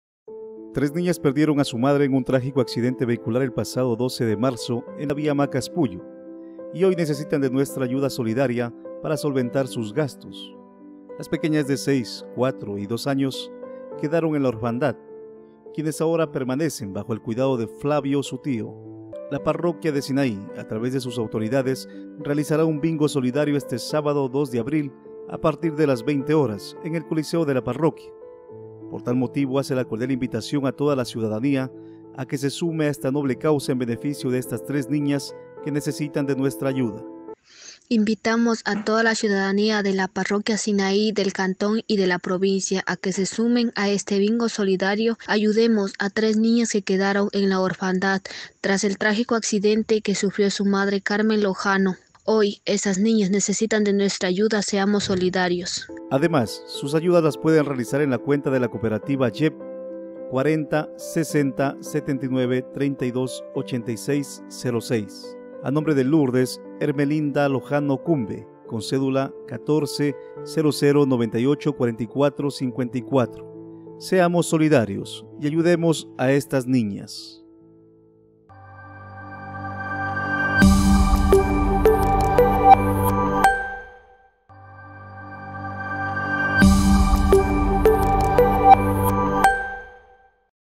NIñas que perdieron a su madre en la parroquia Sinaí, requiere ayuda de Morona Santaigo
[SOLIDARIDAD] MoronaSantiago, tres niñas que perdieron a su madre en un trágico accidente de tránsito, necesitan nuestra ayuda 💕

El pasado 12 de marzo del 2022 un accidente de tránsito registrado en la vía Macas – Puyo, le quitó la vida a Carmen Lojano, dejando en la orfandad a tres niñas de 6, 4 y 2 años de edad.

Actualmente Flavio Lojano tío de las niñas les brinda el cuidadoque estas pequeñas necesitan, sin embargo, con el objetivo de apoyar en el desarrollo de las pequeñas el comité de reinas de la parroquia Sinaí desarrollará este día sábado 02 de abril un bingo solidario a partir de las 20:00 horas en el coliseo de la localidad, para recaudar fondos y ayudar en el desarrollo de esta familia.

Se realiza una cordial invitación a las personas solidarias a sumarse a esta noble causa en ayuda de estas niñas.

Si usted desea colaborar también lo puede realizar a través de la siguiente cuenta de ahorros.

COOPERATIVA JEP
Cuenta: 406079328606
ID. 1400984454
Lourdes Hermelinda Lojano Cumbe